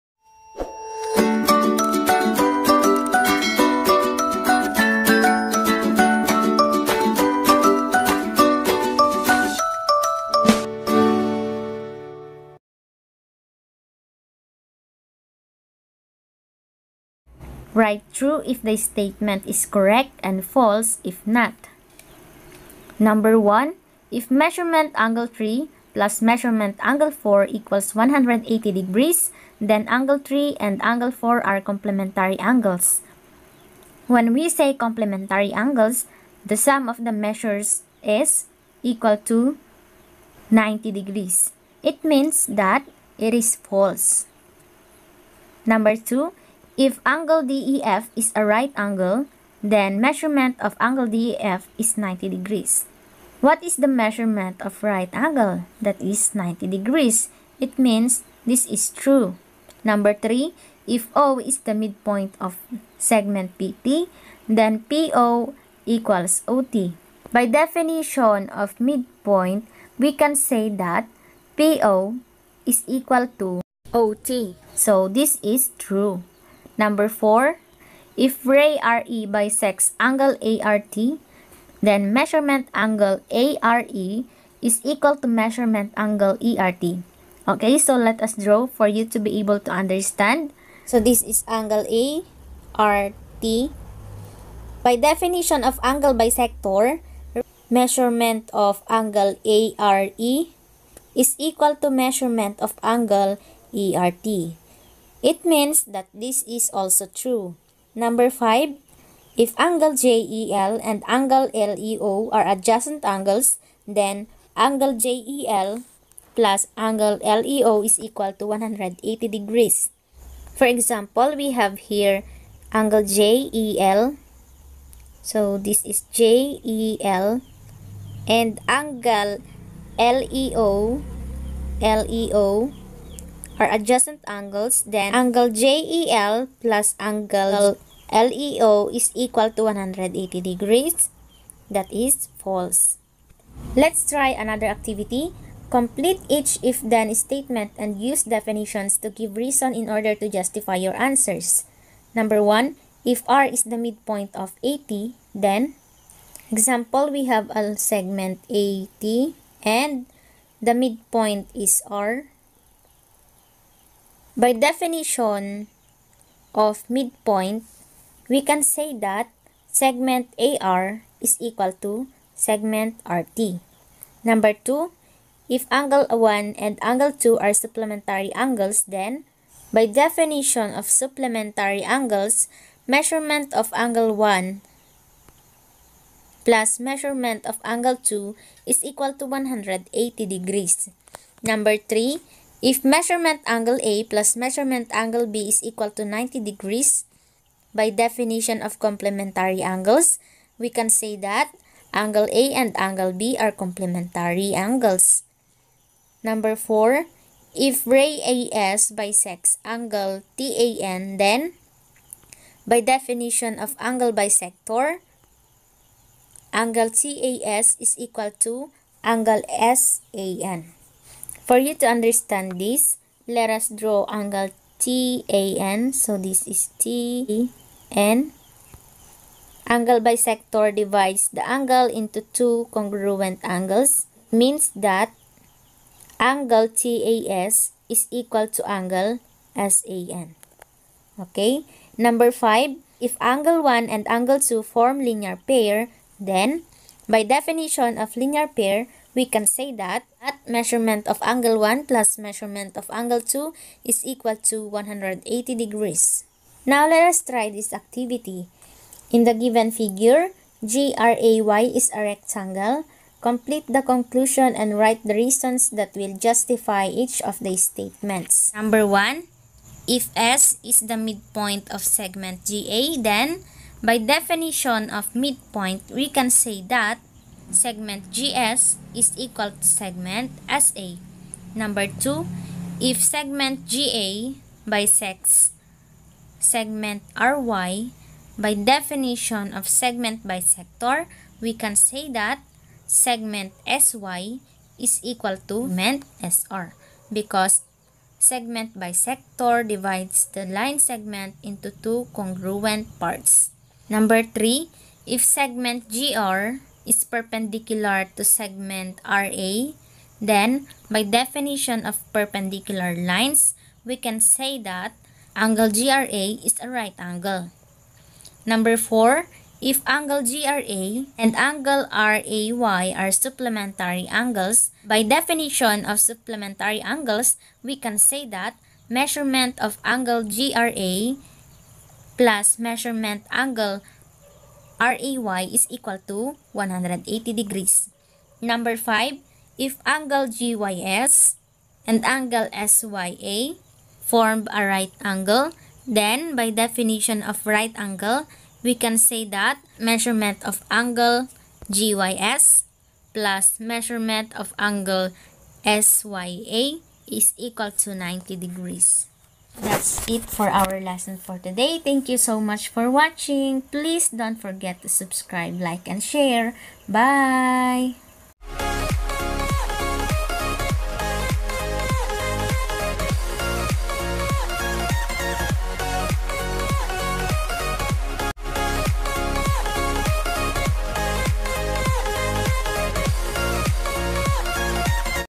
DEFINED TERMS Grade 8 Module 2 Activities Answers and Solutions | English
Grade 8 Module 2 Answers and Solutions
Mathematical System: Defined Terms
1. Definition of Parallel Lines
2. Definition of Perpendicular Lines
3. Definition of Midpoint
4. Definition of Angle
5. Definition of Right Angle
6. Definition of Vertical Angles
7. Definition of Adjacent Angles
8. Definition of Linear Pair
9. Definition of Supplementary Angles
10. Definition of Complementary angles
11. Angle Bisector
12. Definition of Segment Bisector